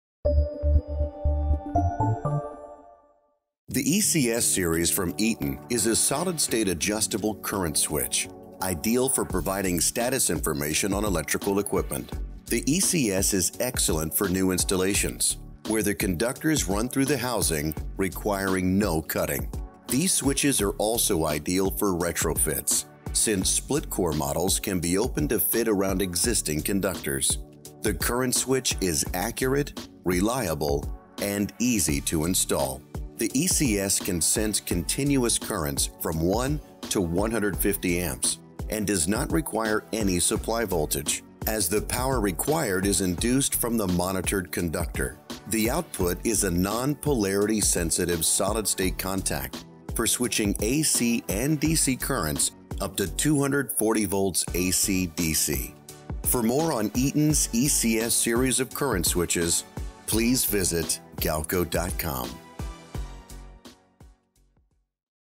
Eaton's ECS Series of Current Switches
The CurrentWatch ECS series from Eaton's electrical business is a family of solid‑state adjustable current switches, ideal for providing status information on electrical equipment. The ECS is excellent for new installations, where the conductors run through the housing, requiring no cutting. These switches are also ideal for retrofits, since split‑core models can be opened to fit around existing conductors. the current switch is accurate, reliable and easy to install.

The ECS can sense continuous currents from 1 to 150A and does not require any supply voltage, as the power required is induced from the monitored conductor. This switch also includes and LED indicating two states: on and below trip point, and above trip point and contacts energized.

Any change in current can be sensed with the ECS Series. A change in current may indicate motor failure, belt loss/slippage or mechanical failure. Any of these events can cause the current to drop significantly, tripping the switch and notifying the controller.